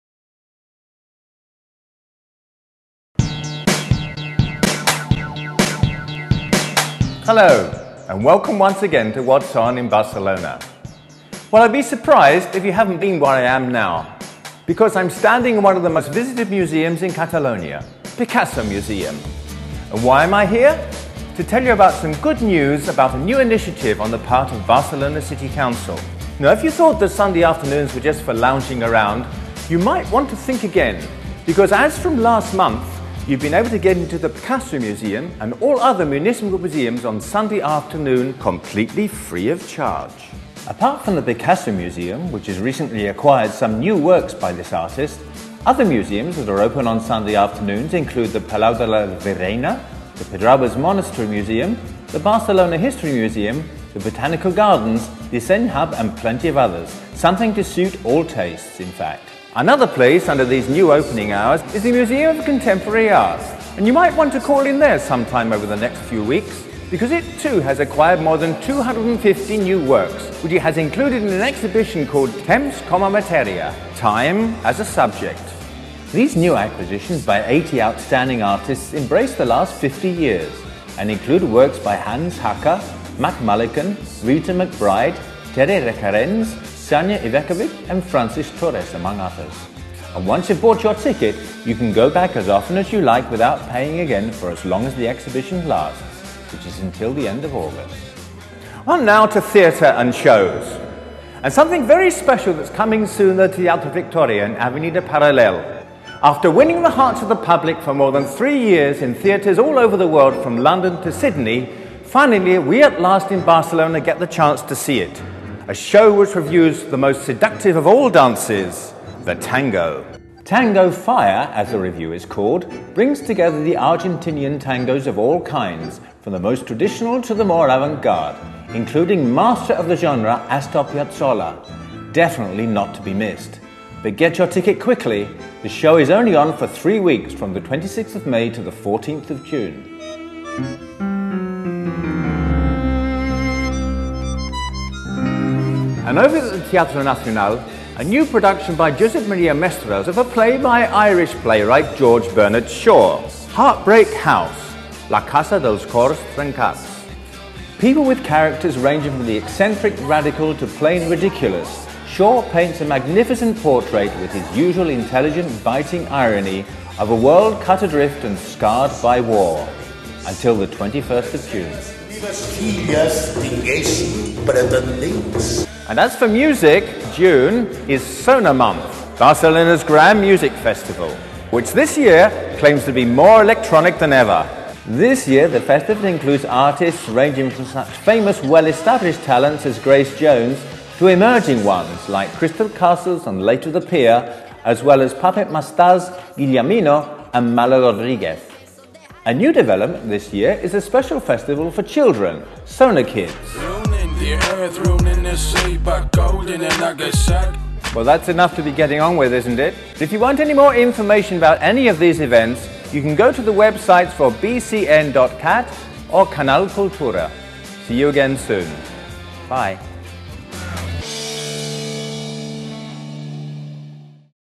What's On in Barcelona, june 2009
In this video section, we'll be recommending some of the best cultural offerings to be found in the city. Don't miss out!